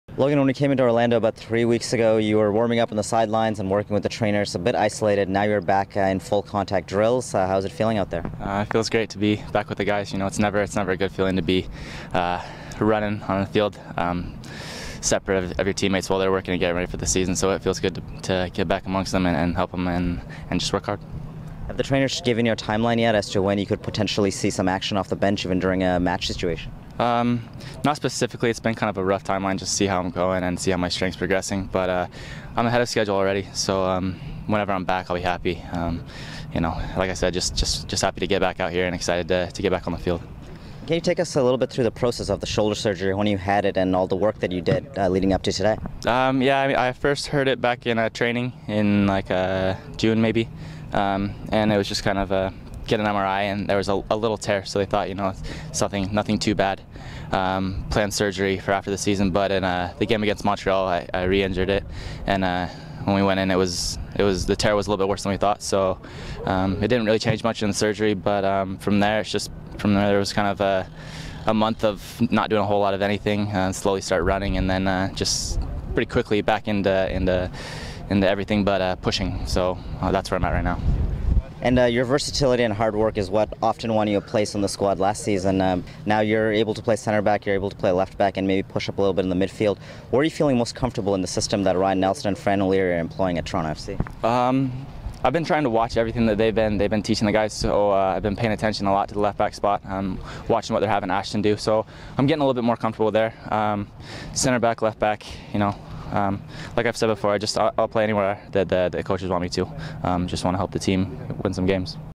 Logan Emory - February 19, 2013
Defender Logan Emory is back to taking part in regular drills at training. Emory had shoulder surgery in the offseason.